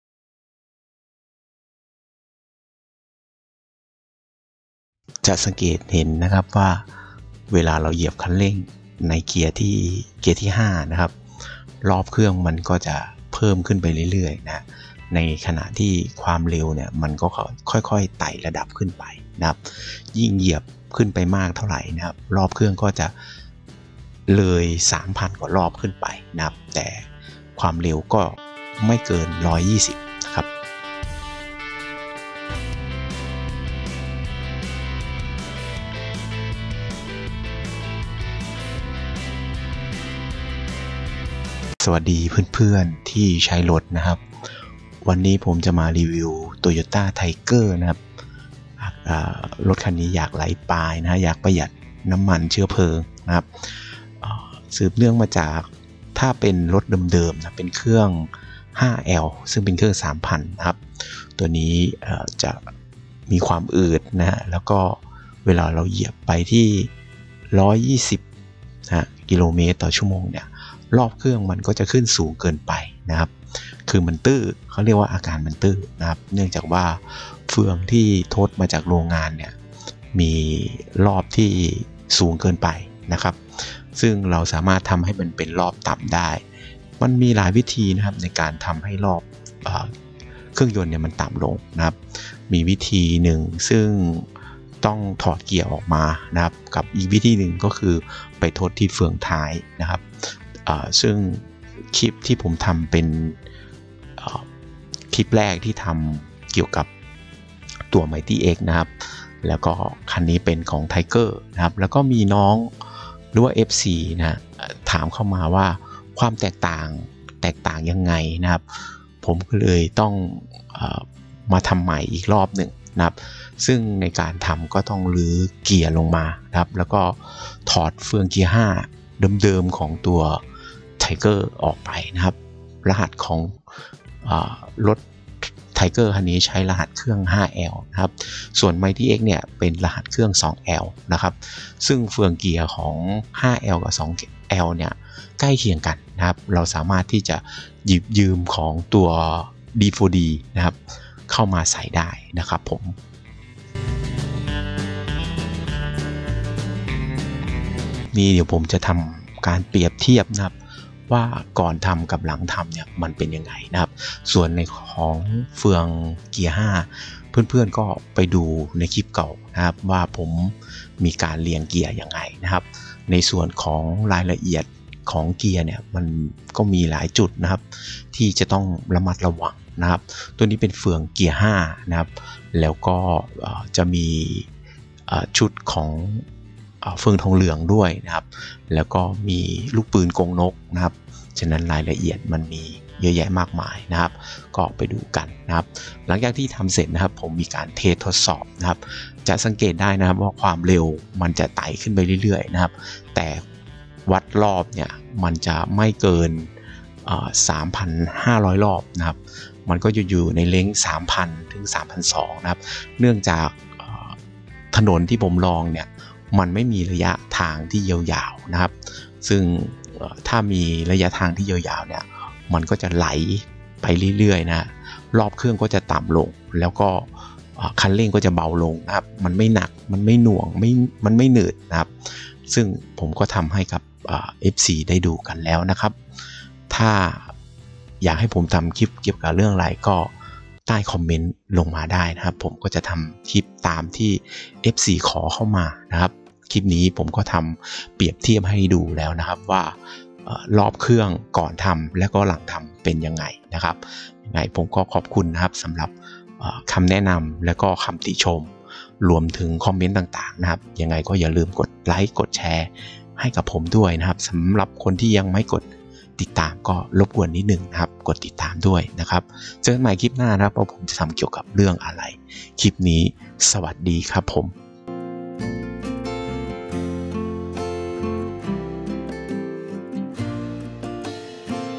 ลดรอบเครื่องยนต์เพิ่มความเร็ว
การทำให้รอบเครื่องยนต์ลดลงไปเพิ่มความเร็ว สามารถทำได้โดยการเปลี่ยนเฟืองท้าย และทำการเปลี่ยนเฟืองเกียร์5 รถโตโยต้า ไทเกอร์5l เครื่องยนต์จะมีความอึดกินน้ำมันเชื้อเพลิง เราสามรถทำให้รถวิ่งดีขึ้น โดยใส่เฟืองเกียร์5 เมธพาไป จะริวิวให้ดู ลดรอบเครื่องยนต์เพิ่มความเร็ว